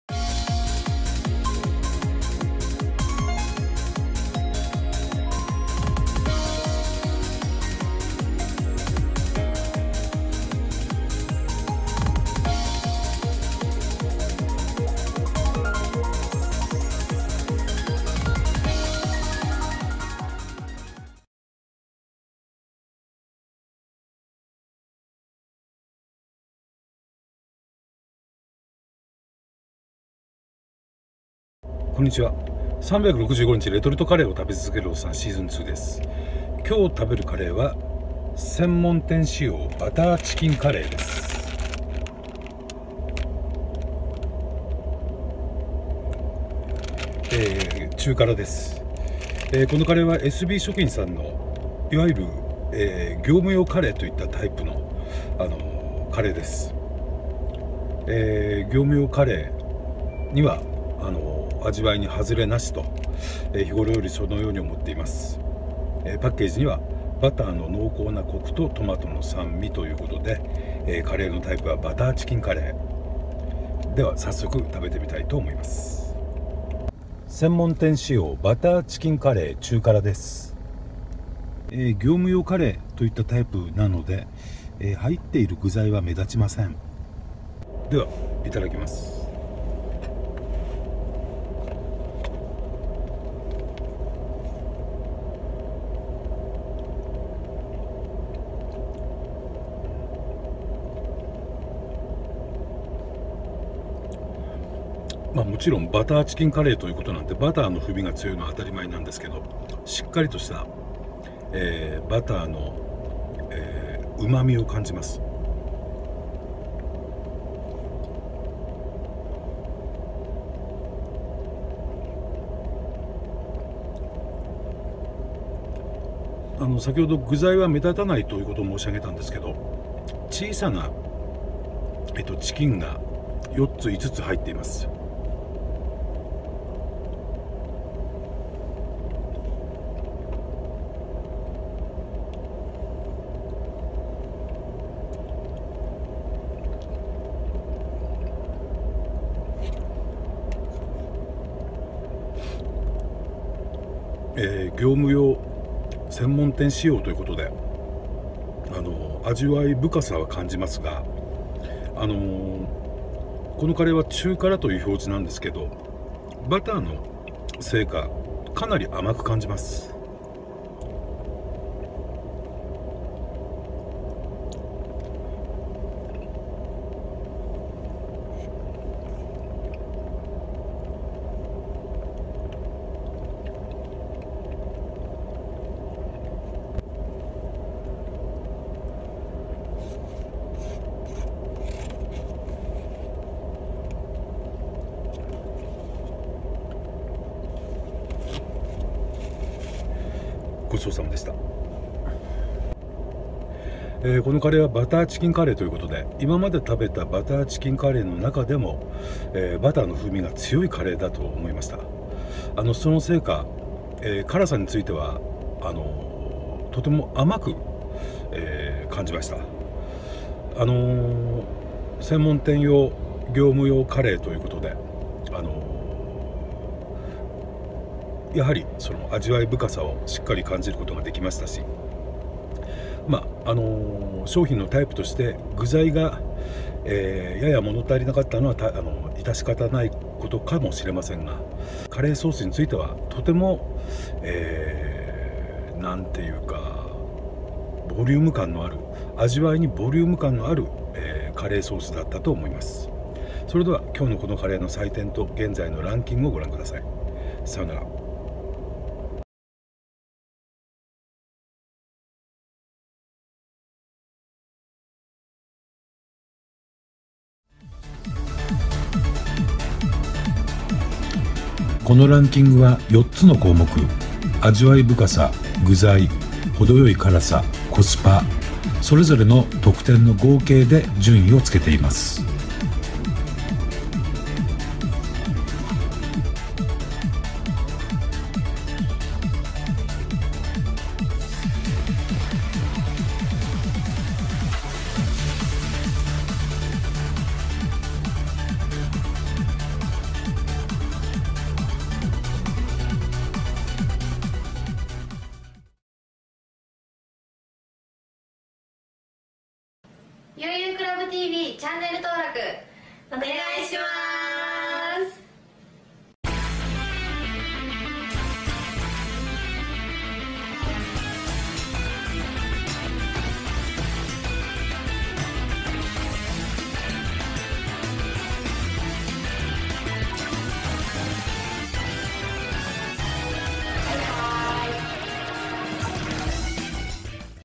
【365日レトルトカレーを食べ続けるオッサン season2】#285 専門店仕様 バターチキンカレー 中辛
2017年10月11日！S&Bの業務用！専門店仕様のカレー！！バターの風味が効いている！！